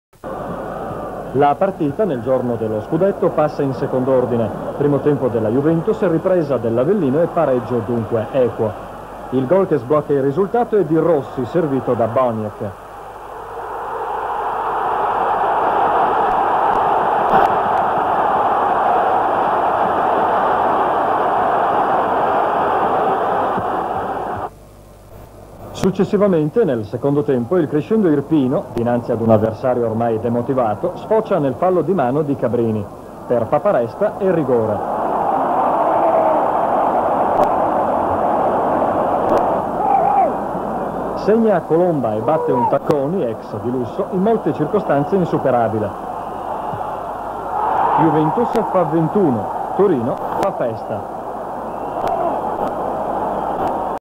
1983/84, Serie A, Juventus - Avellino 1-1 (29)
Nel Campionato di Serie A del 1983/84 nella quattordicesima giornata del girone di Ritorno, Juventus - Avellino finisce 1-1.